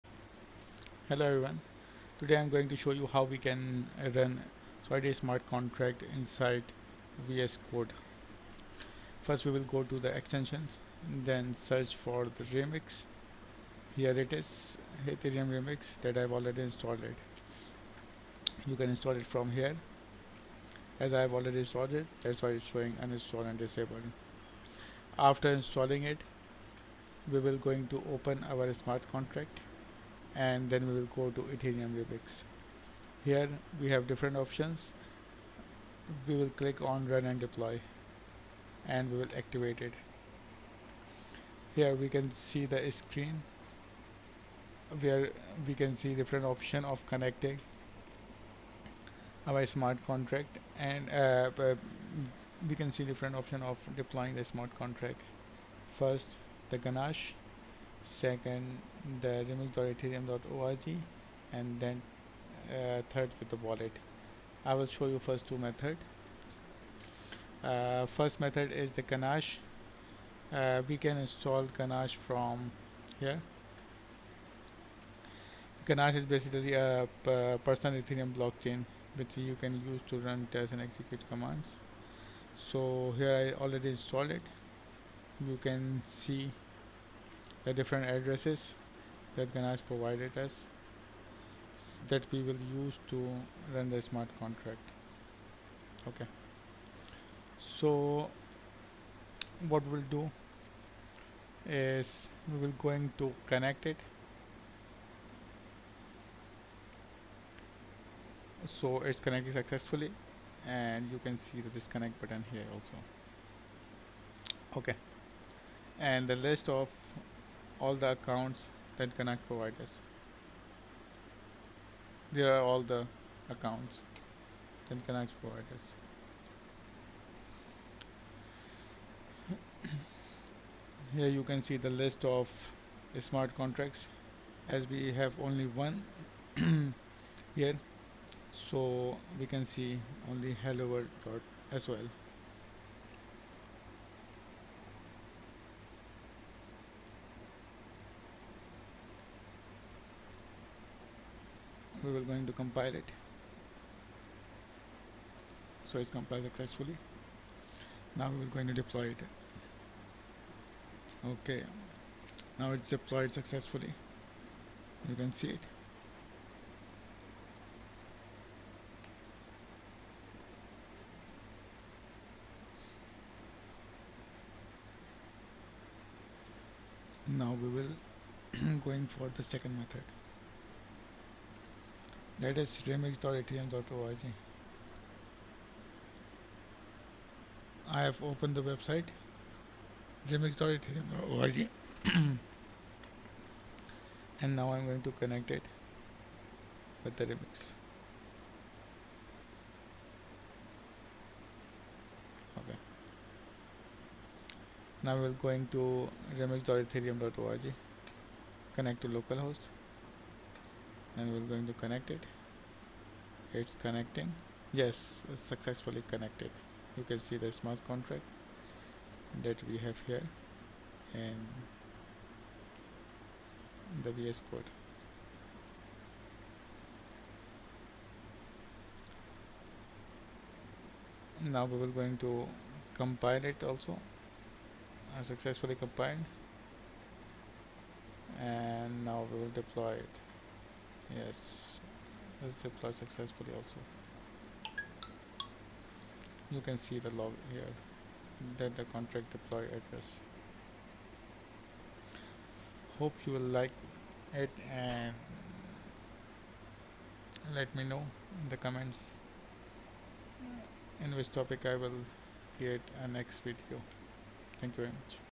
Deploy Solidity Smarts Contract Using VSCode Extension | Etherum blockchain
In this video, I have shown you how to deploy solidity smart contracts using VSCode IDE.

vscode remix smartcontract

smart contracts, Ethereum, blockchain, programming, vscode